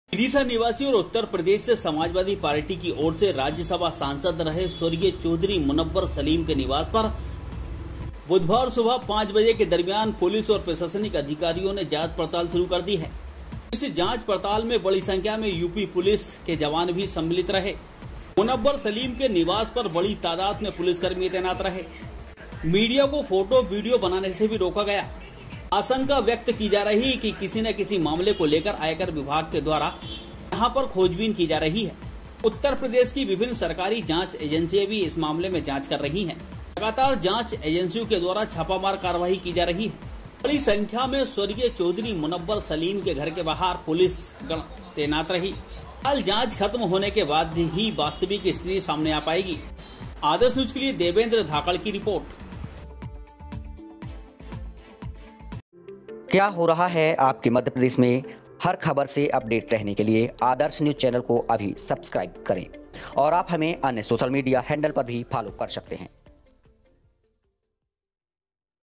vidish। सपा सांसद रहे मुनव्वर सलीम के घर छापामार कार्यवाही।यूपी पुलिस रही तैनात
Video from Adarsh News MP